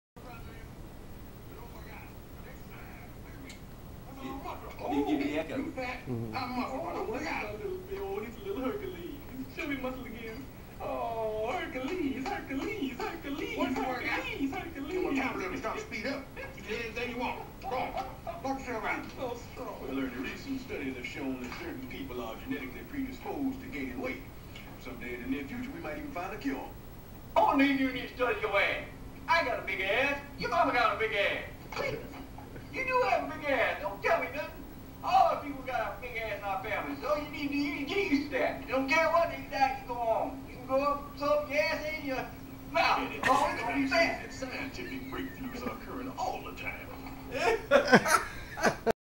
Richard as Cletus_0001.wmv
Richard L. Rigdon playing Cletus from The Nutty Professor